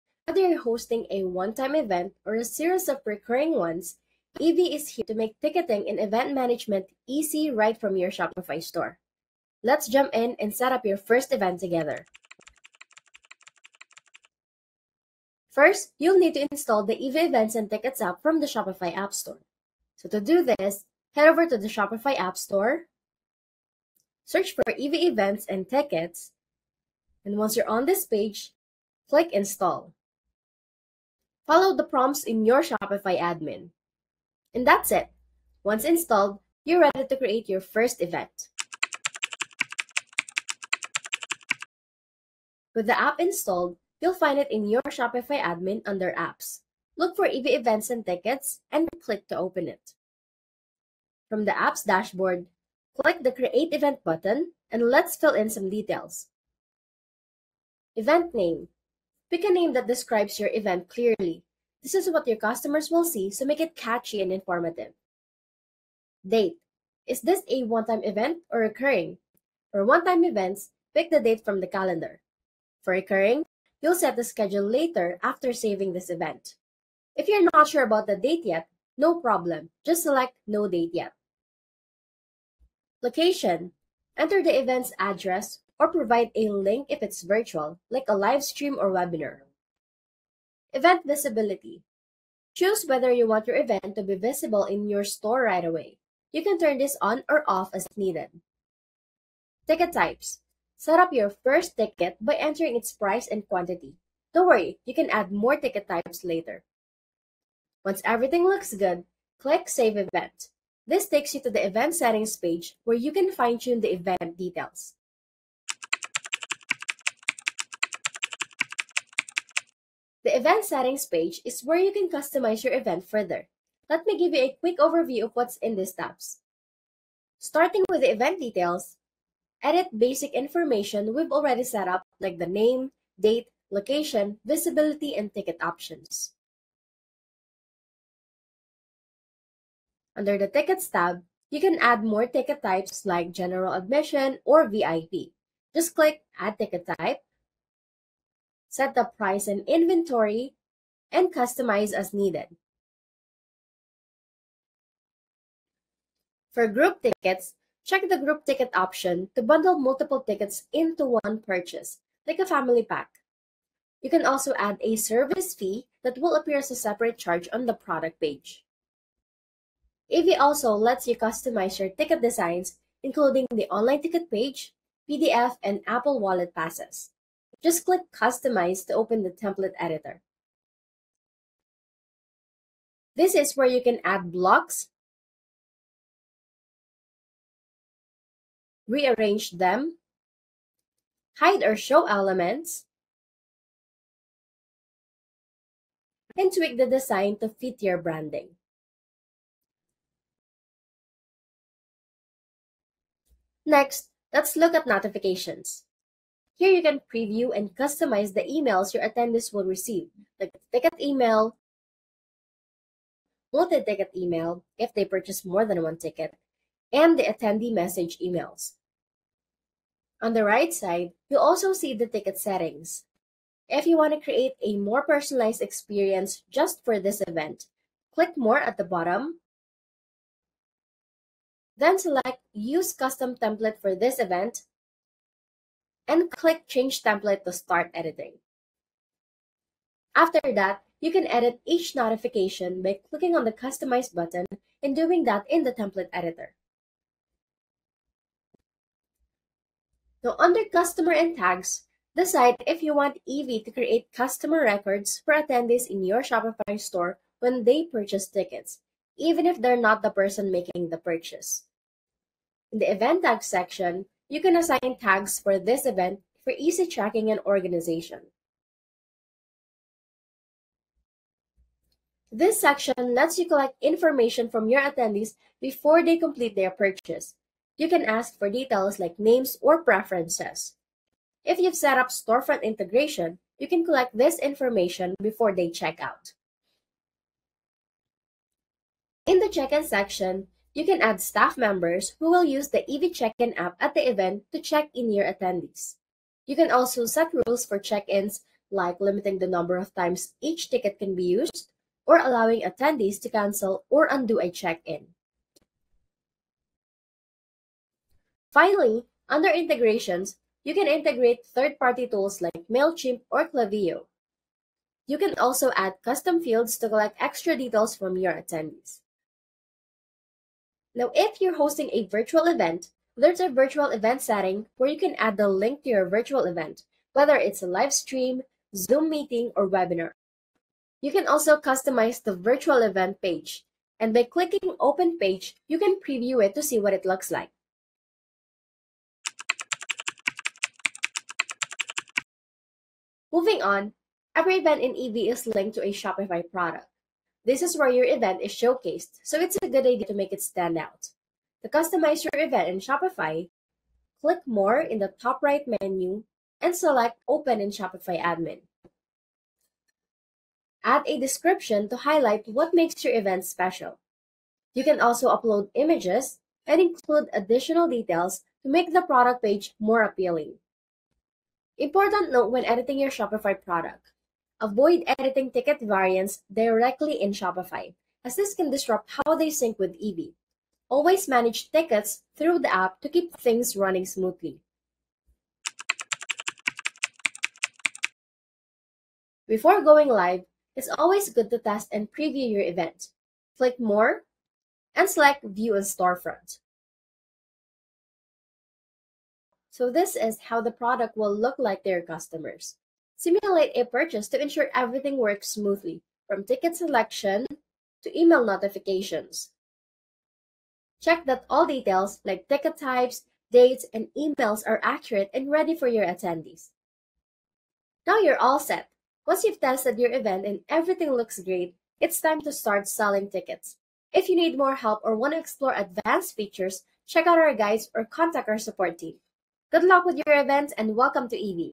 How to Set Up Your First Event with Evey: A Complete Step-by-Step Guide
Ready to host your first event with Evey? This guide takes you through each step, from installing the app to creating and customizing your first event. Learn how to set up tickets, adjust settings, and tailor your event to fit your needs. Perfect for new event organizers looking to get started with Evey quickly and easily!